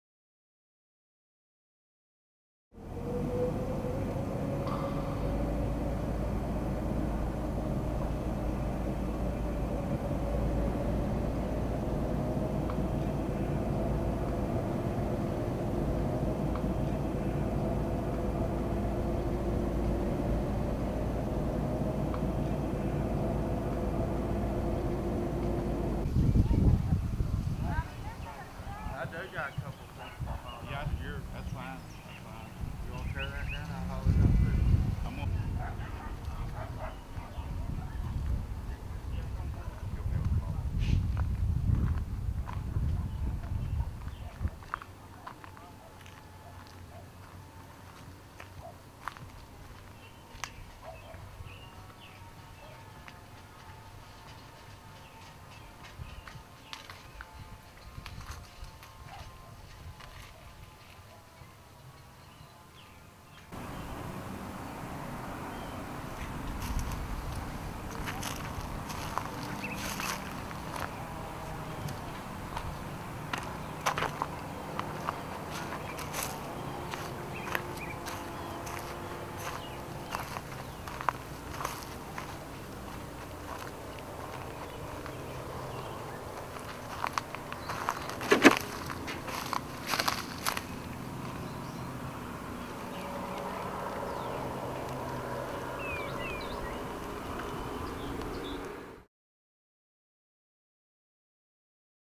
Cleveland Tornado Destruction - Bell Road Area
Cleveland Media Network - Ccast Community (4/29/11). Destruction caused by the April 27th tornados in Cleveland and Bradley County, Tennessee.